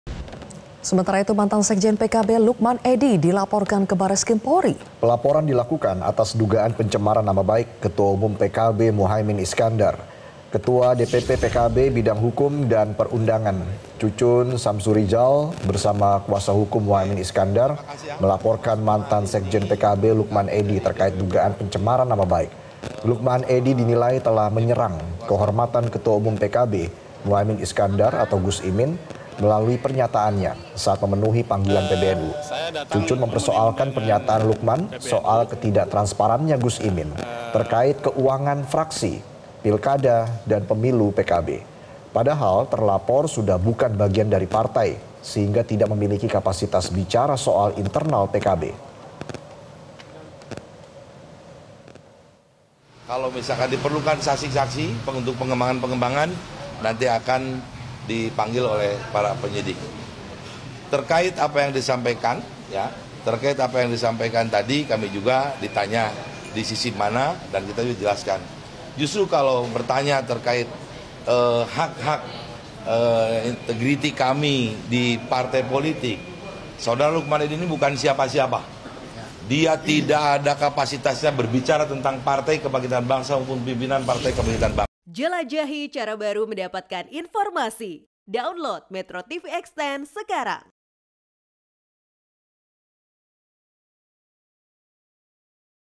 Pencemaran Nama Baik, Mantan Sekjen PKB Dipolisikan
MetroTV, Mantan sekjen PKB, Lukman Edy dilaporkan ke Bareskrim Polri. Pelaporan dilakukan atas dugaan pencemaran nama baik, Ketua Umum PKB Muhaimin Iskandar